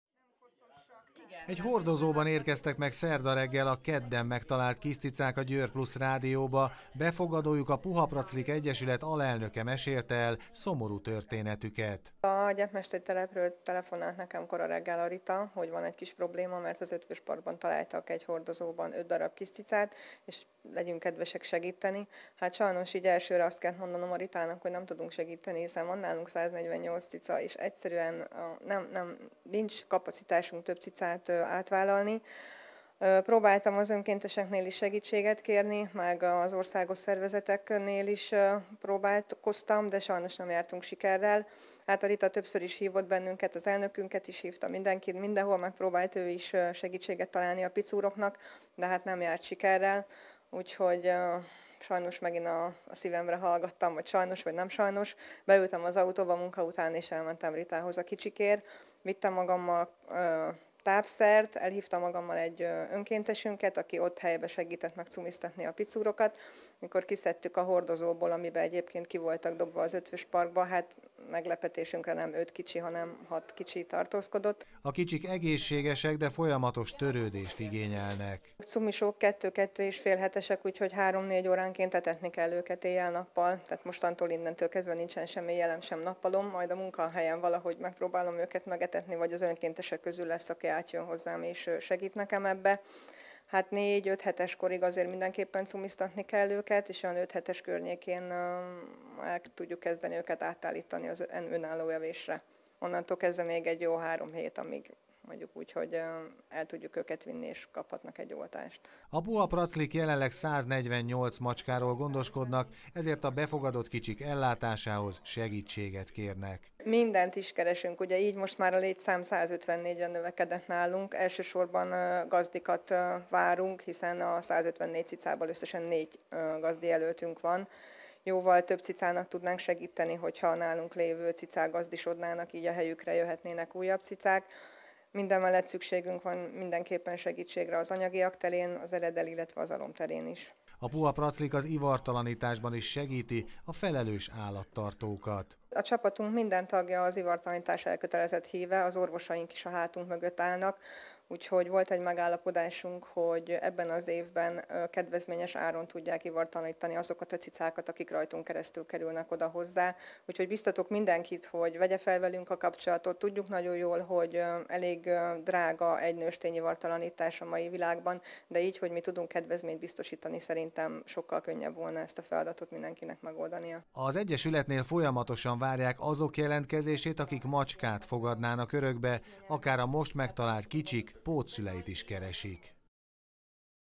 Hat kiscicát hagytak sorsukra: segítséget kérnek
Hat kiscicát hagyott sorsára valaki kedden egy győri parkban, a kicsiket előbb a gyepmesteri telepre vitték, majd a Puha Praclik Egyesület fogadta be az ártatlan szőrmókokat. A kicsik nagyjából két és fél hetesek lehetnek, egészségesek, de három óránként cumiból kell őket etetni. Az egyesület jelenleg 148 macska ellátásról gondoskodik, ezért komoly teher számukra a kiscicák befogadása és nevelése, amihez segítséget kérnek a jóérzésű emberektől. A Puha Praclik a macskák ivartalanításában is segít a felelős állattartóknak.